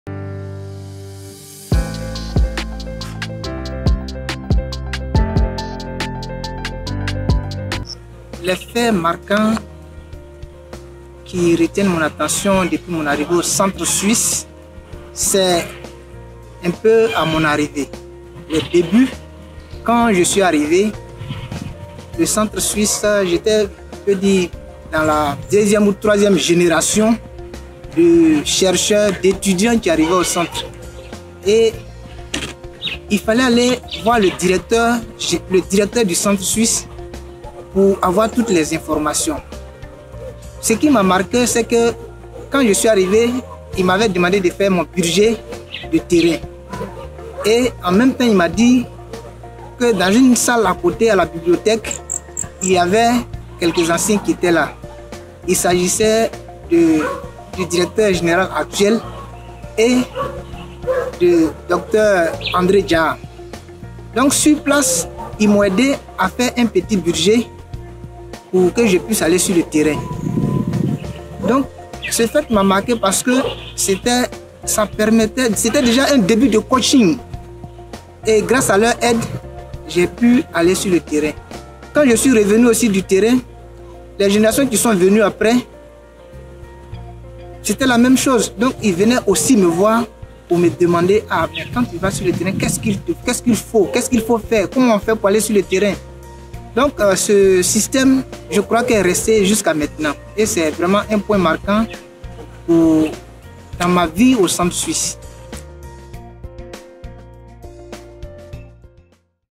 SERIE CAPSULE "SOUVENIR SOUVENIR" –Dr Goné Bi Zoro -
Docteur Goné Bi Zoro Bertin, Chercheur associé au  Centre Suisse de Recherches Scientifiques en Côte d'Ivoire et Enseignant - Chercheur à l' Université Félix Houphouët-Boigny Officiel est notre intervenant dans la capsule « SOUVENIR SOUVENIR » du jour.
Il explique comment il a été marqué par cet esprit d’entraide, de partage de connaissance qui règne au Centre Suisse, et ce, depuis son intégration dans l'Institution.
Ecoutons-le !😊